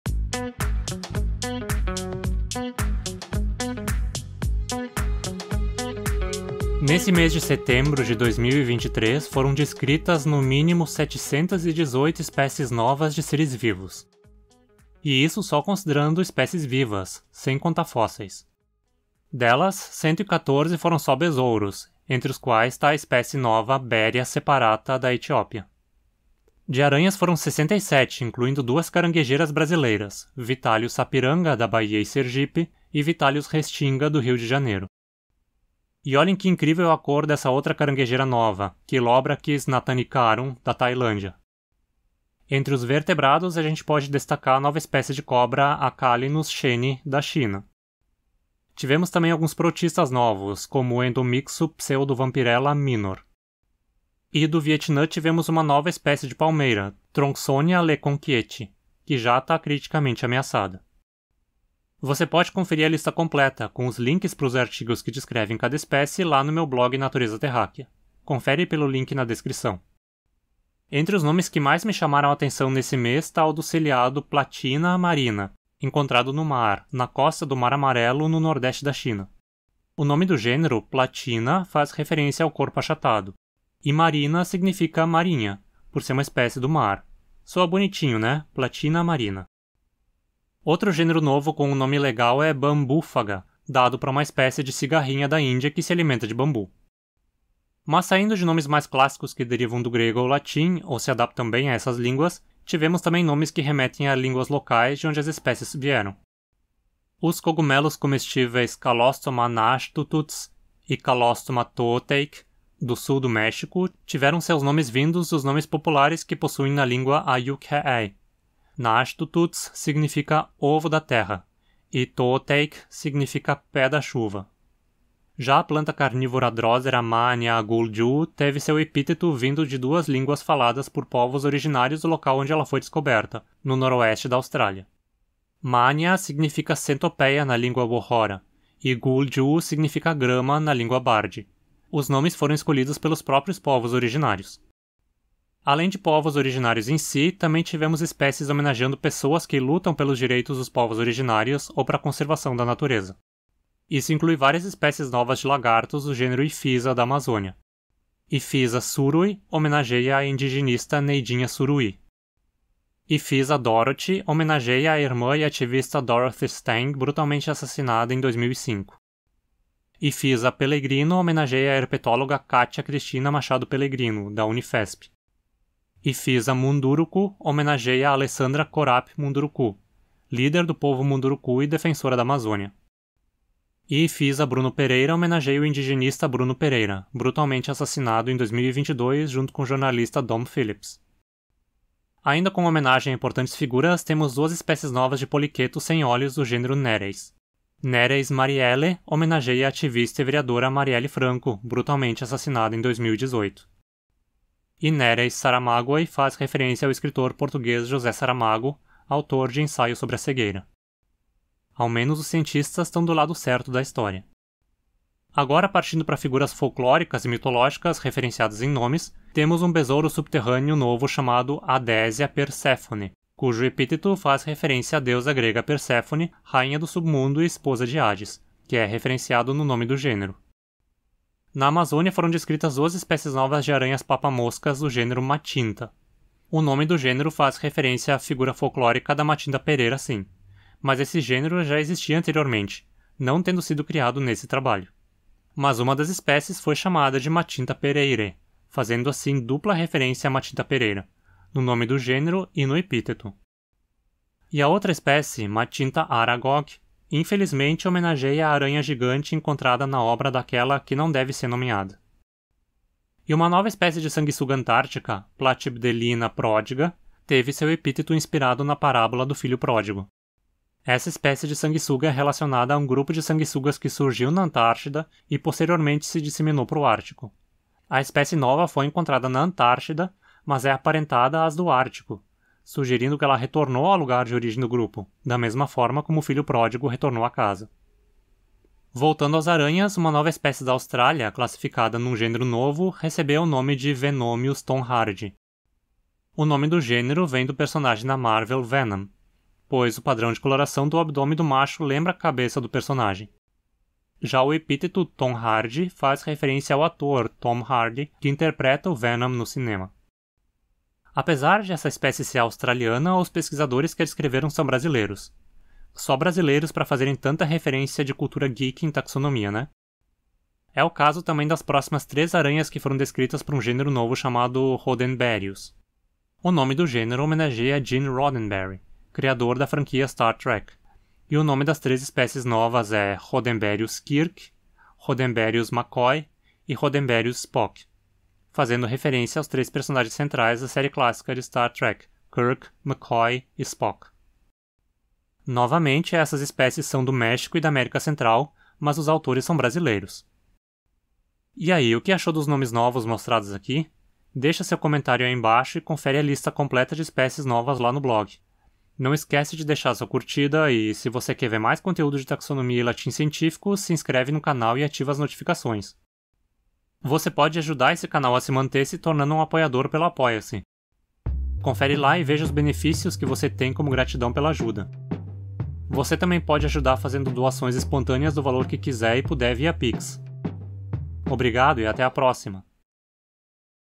Os melhores (ou piores?) nomes científicos: taxonomia em setembro de 2023
Pelo menos 718 espécies novas foram descritas em setembro de 2023. Vamos ver um apanhado geral das mais interessantes, a meu ver, nesse vídeo.

O uso de imagens com direitos autorais neste vídeo tem fins de estudo, crítica ou polêmica e obedece o estabelecido no Art. 46 da Lei Nº 9.610 (Lei de Direitos Autorais).